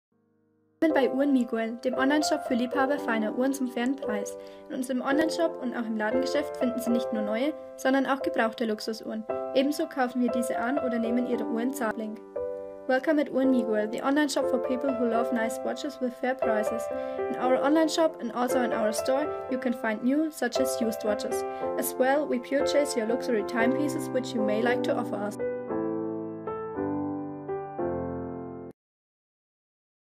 Anonimo Nautilo Automatic FM13051
Anonimo Nautilo Automatic

Referenz:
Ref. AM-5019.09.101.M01
Geschlecht:
Herrenuhr
Gehäuse:
Edelstahl, Durchmesser ca. 42 mm, Höhe ca. 12 mm, verschraubter Glasboden und Krone, wasserdicht bis 20 atm, Drehlünette mit Keramik